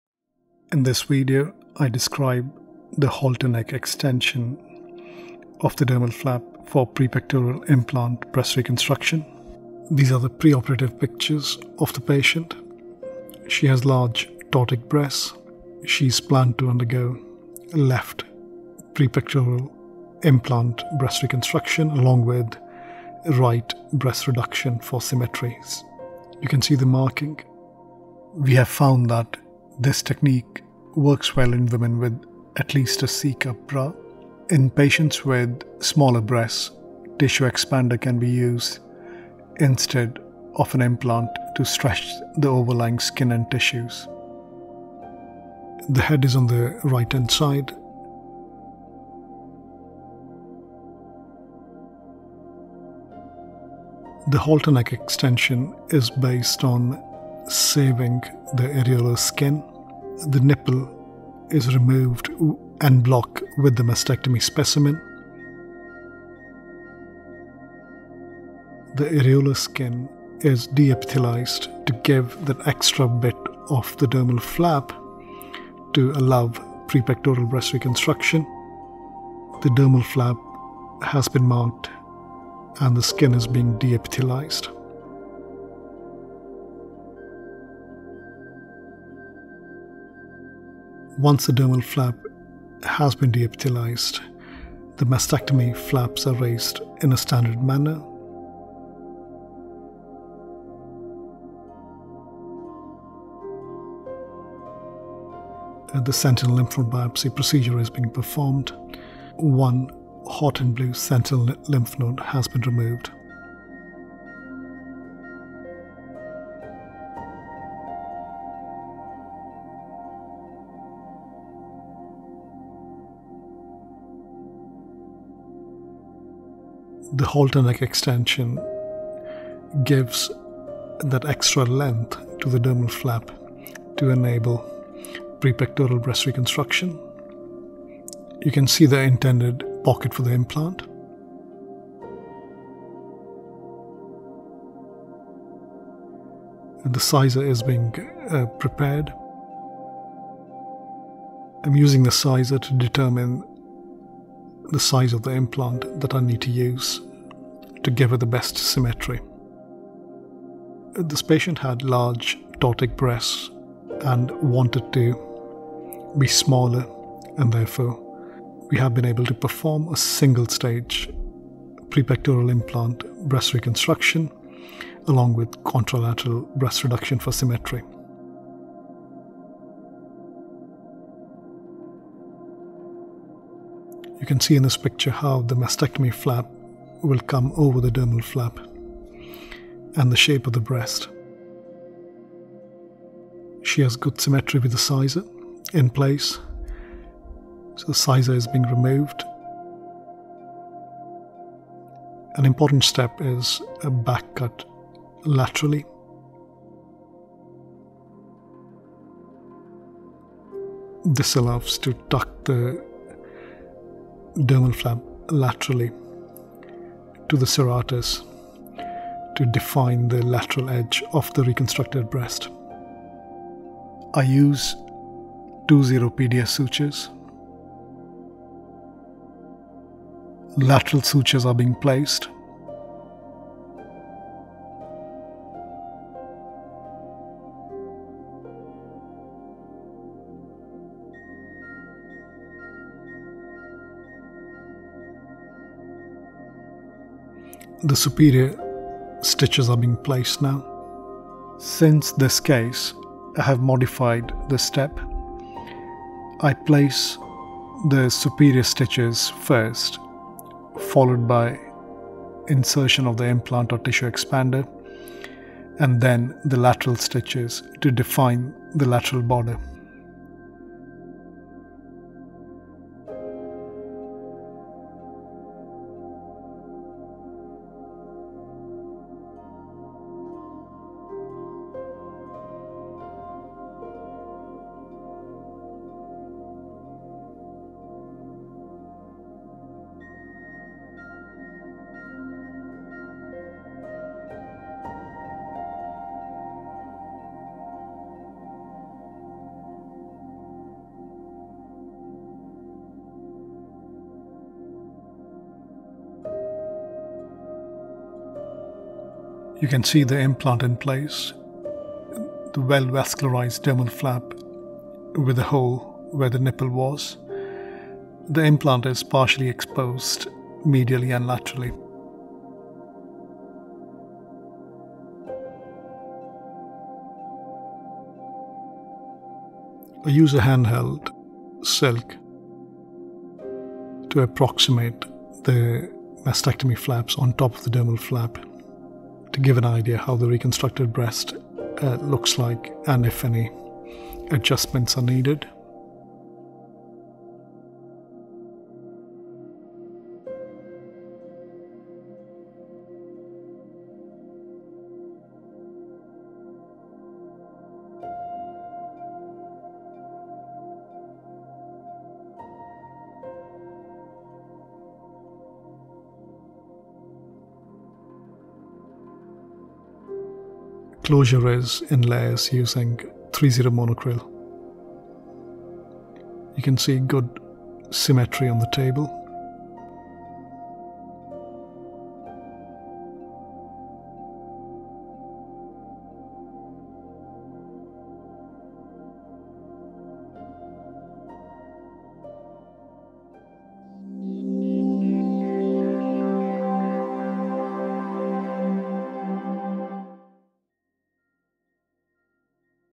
Halter neck extension of dermal flap for prepectoral implant breast reconstruction
The video describes the 'halter neck' modification of dermal flap for prepectoral implant breast reconstruction.